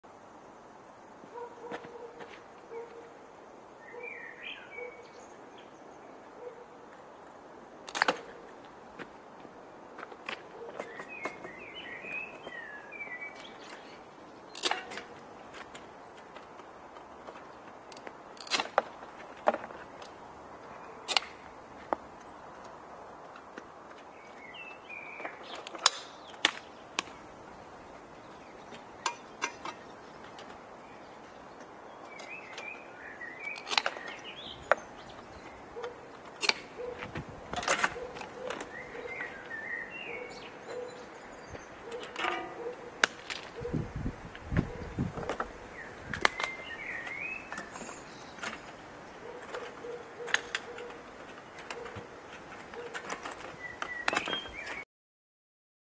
If – 6 vajerlås kapas på 30 sekunder
Alla lås håller inte måttet, här kapas 6 vajerlås på 30 sekunder. Skaffa lås godkänt av Svenska Stöldskyddsföreningen. Läs mer